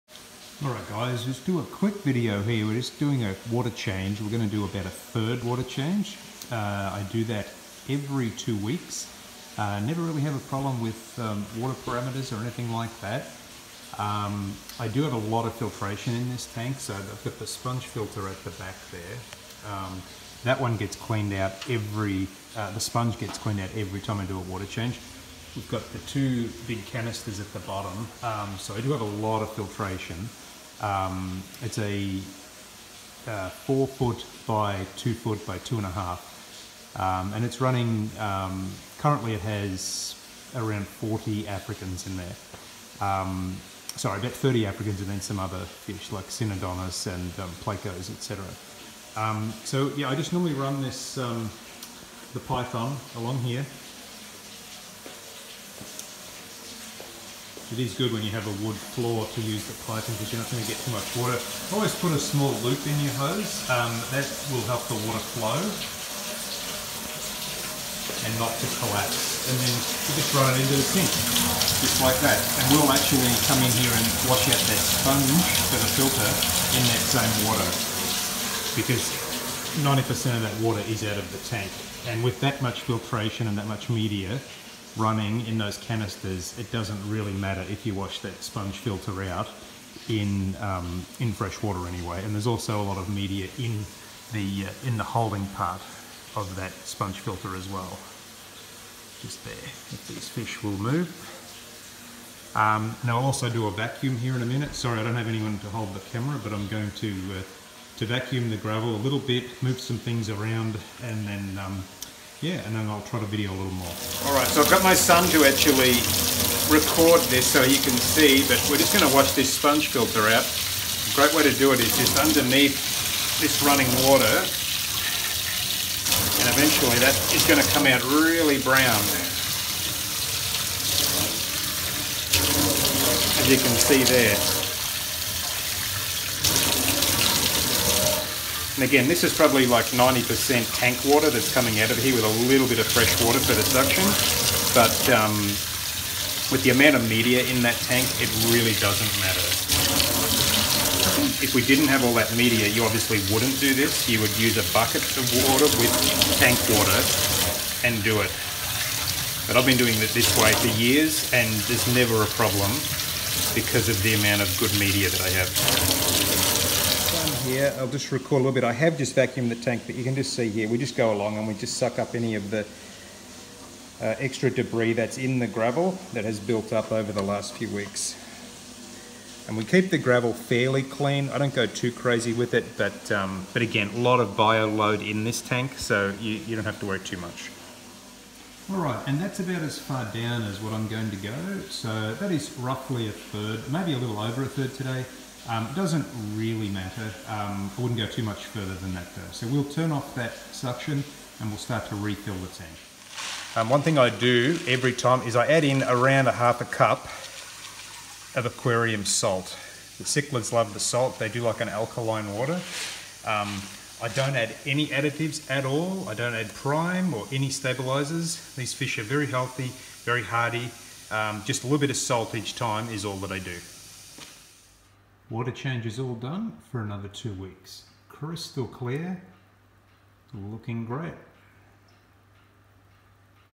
Cleaning the Cichlid Tank Review | How to Clean Out a Fish Tank
This large African Cichlid tank gets cleaned out every two weeks. I've been keeping Cichlid's for over 20 years now.